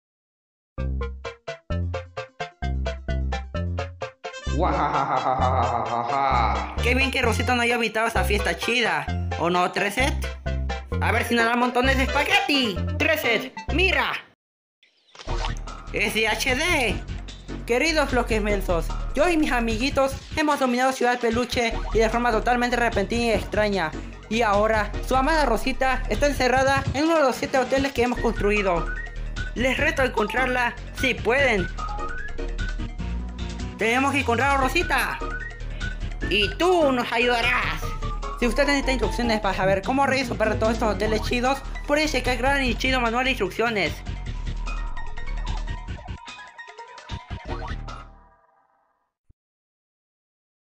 Hotel Rusty ¡Re-alquilado! (ESPAÑOL)
Gracias He nry xd

Un remake chido de una animación parodia mía de la intro del nefasto Hotel Mario (1994) un juego digno de memes.

*memorias de los ytph*